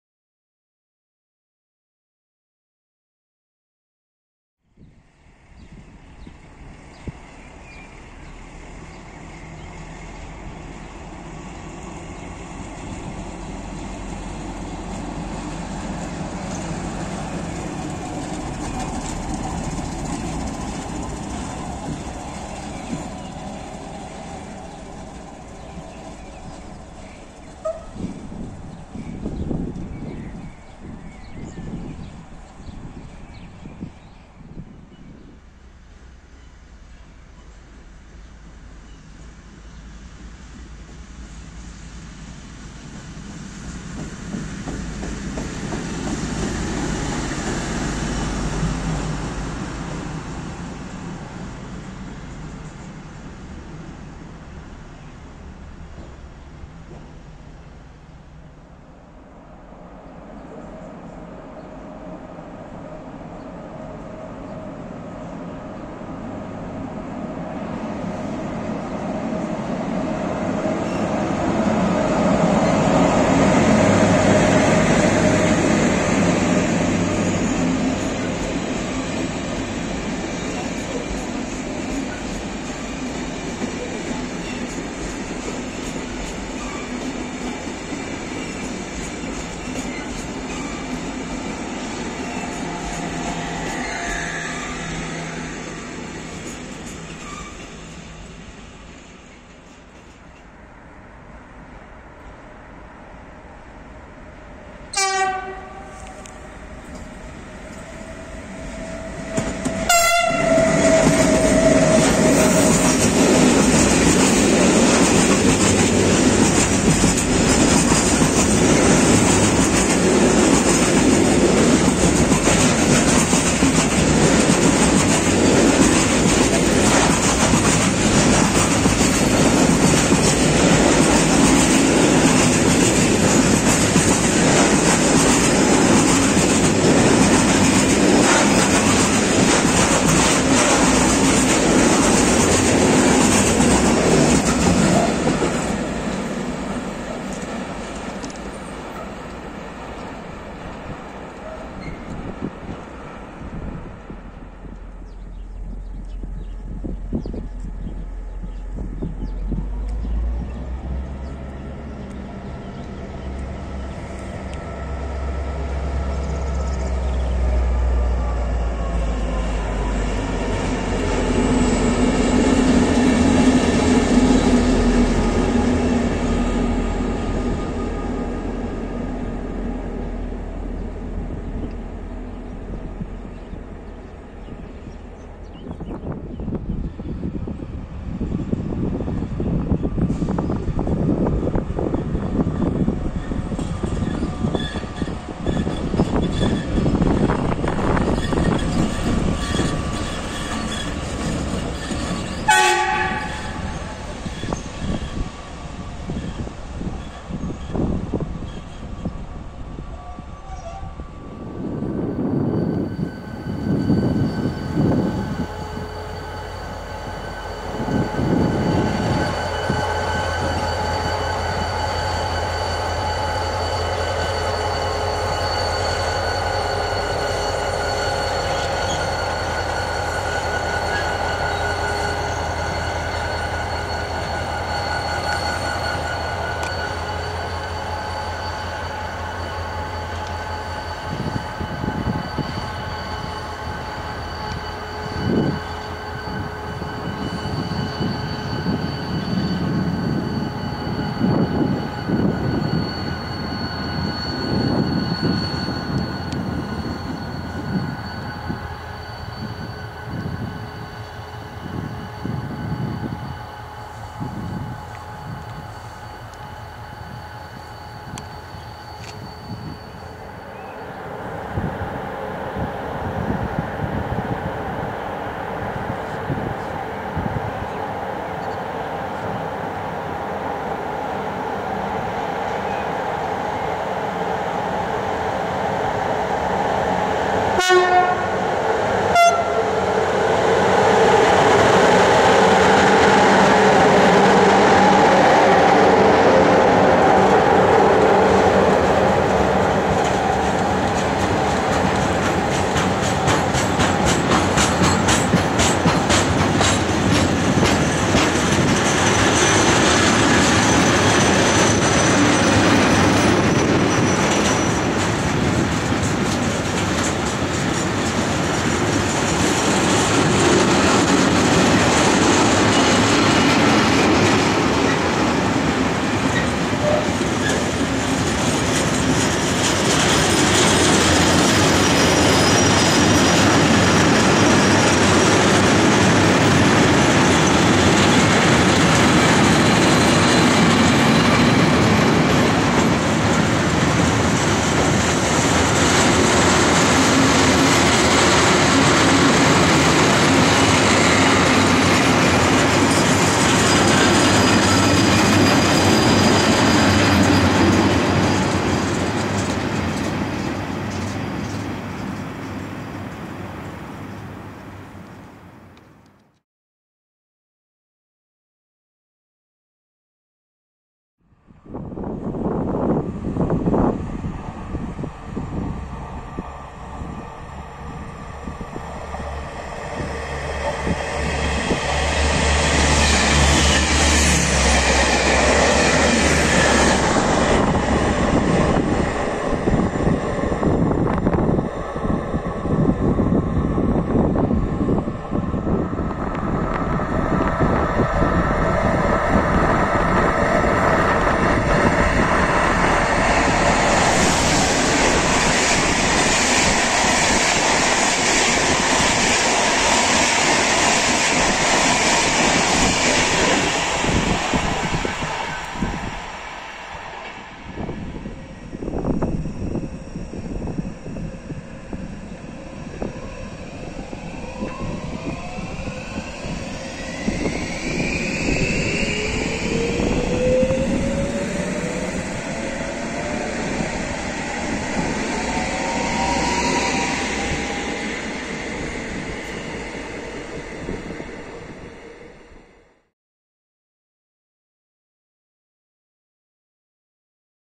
Rail traffic in Zagreb, Croatia. Željeznički promet u Zagrebu, Hrvatskoj.
Enjoy in the video!
Uživajte u videu!
Greetings to all train engineers and railfans!
Pozdravi svim ljubiteljima željeznice i strojovođama!

Snimano sa: Redmi Note 9 Pro, iPhone 12 i Sony HDR-CX155E.
Recorded with: Redmi Note 9 Pro, iPhone 12 and Sony HDR-CX155E.